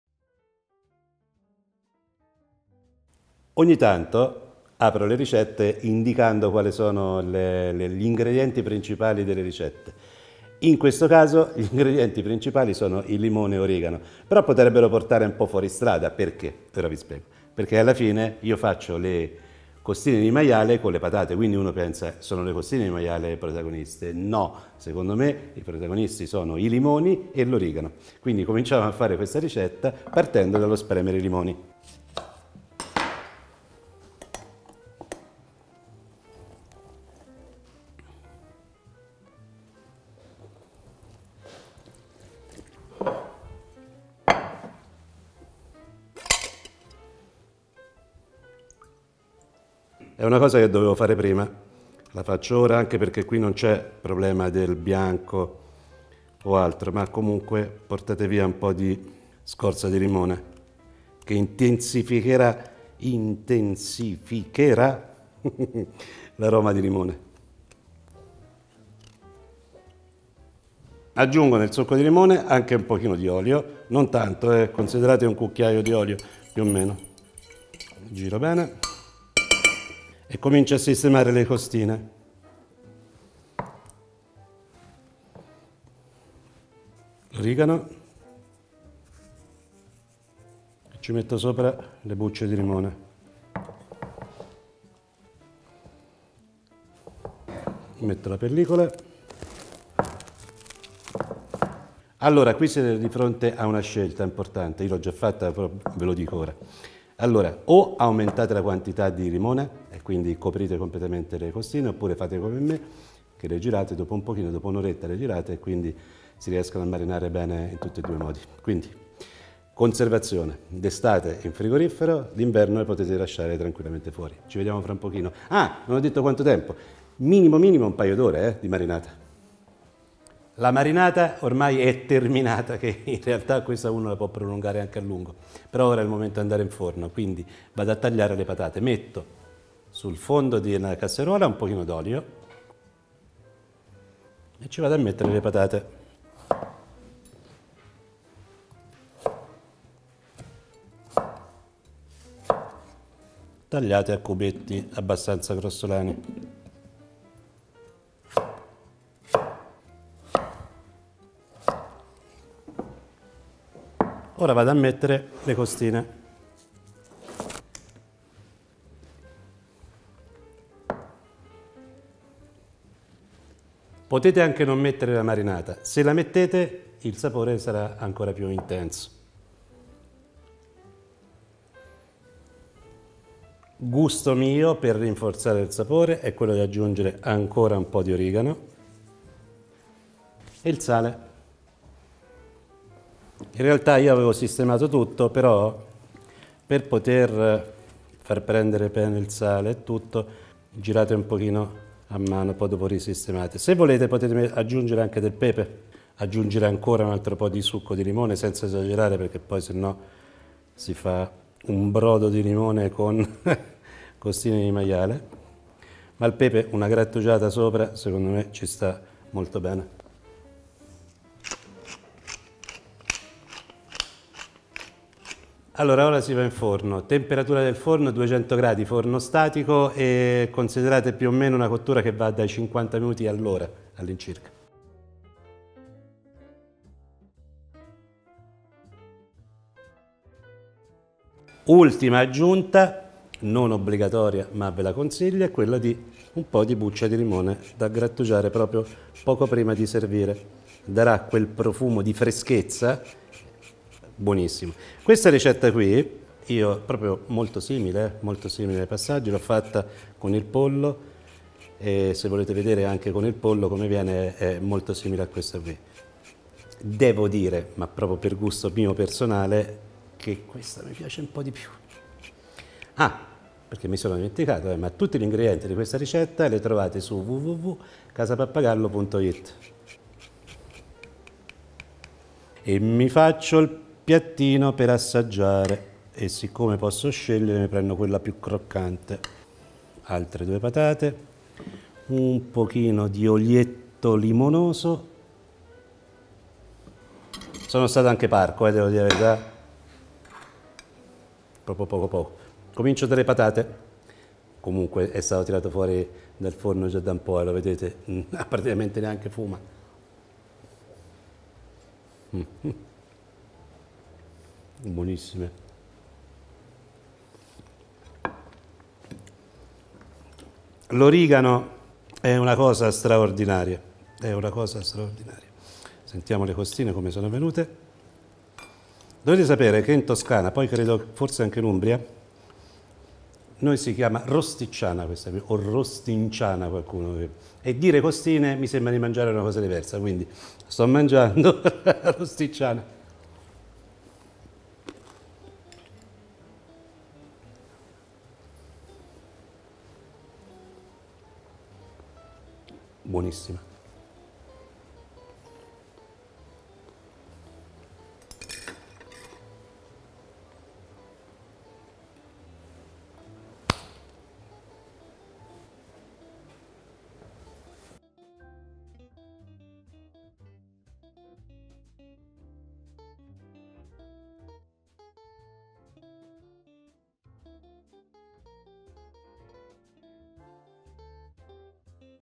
Costine di maiale al limone con patate e origano S3 - P60
Le costine di maiale al limone e origano con patate sono un secondo piatto delizioso, semplice da fare e molto aromatico vista la presenza degli agrumi e di questa profumatissima erba aromatica.

Una ricetta molto furba in quanto in un colpo solo avrete secondo e contorno e quando porterete a tavola sarà festa per tutti, in quando piacerà a grandi e piccoli.

La presenza del limone va a contrastare in maniera decisa il sapore dolce della carne di maiale, la marinatura inoltre ne assicura morbidezza e succosità.

In questo caso abbiamo utilizzato la carne di maiale ma sappiate che questa ricetta può essere fatta anche con l’agnello, il manzo e perché no, anche con i fuselli di pollo.

► Ingredienti per preparare le costine di maiale al limone con patate e origano:
Costine di maiale 1 kg
Limoni 4
Patate 1 kg
Origano q.b.
Olio EVO q.b.
Sale q.b.